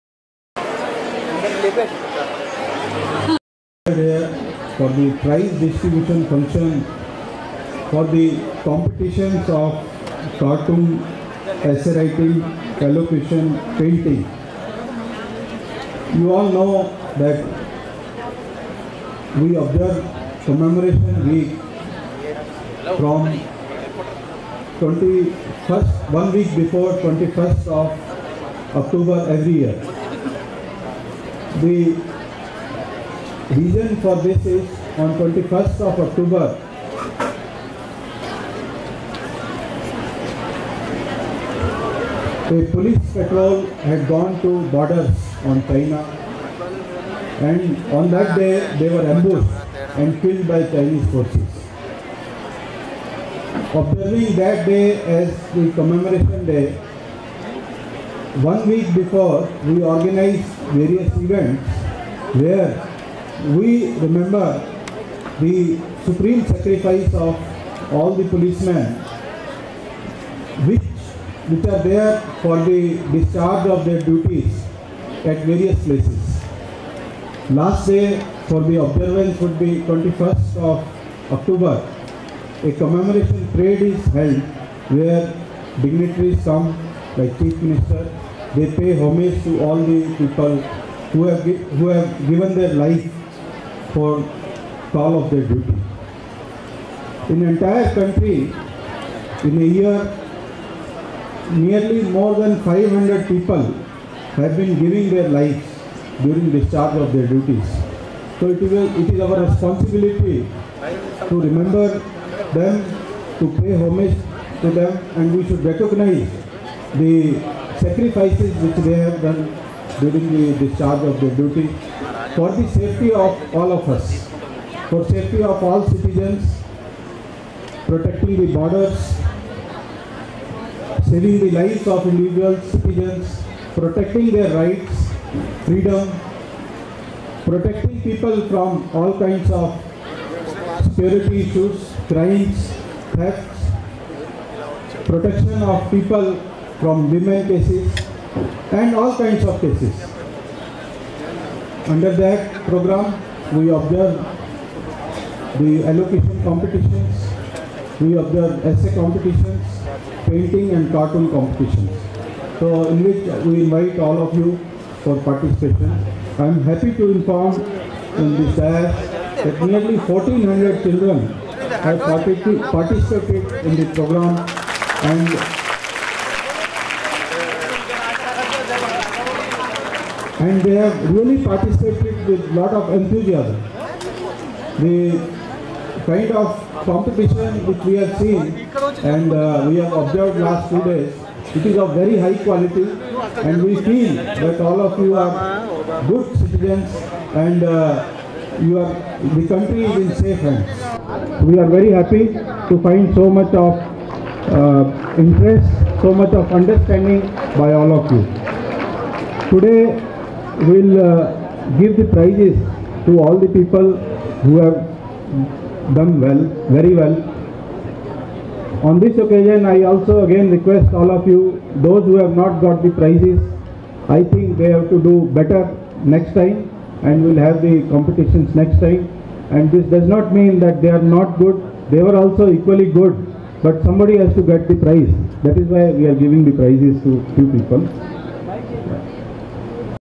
Addl.CP. Traffic prize distritubiton ceremony speech/PRO HYDERABADPOLICE
As part of Police Commemoration Week competitions were conducted for School Children in Painting, Cartoon, Essay and Elocution. Prize distribution ceremony conducted at Indira Priyadarshini Auditorium, Public Garden on Today (18th October, 2016) at 4pm Sri.M.Mahendar Reddy,IPS, C.P.Hyderabad  will  attend as chief guest  and also All senior officers of Hyderabad city also attend the program